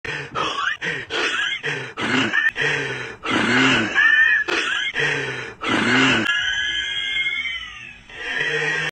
This is how I feel when I’m unloading paintings after a show!
Artist Life |  Edgar Medina

It’s been inspiring to take the studio on the road! Taking the time to get inspired by my homeland and the beautiful landscapes. Today, I am doing a second live painting now from Holbox 🇲🇽👨🏻‍🎨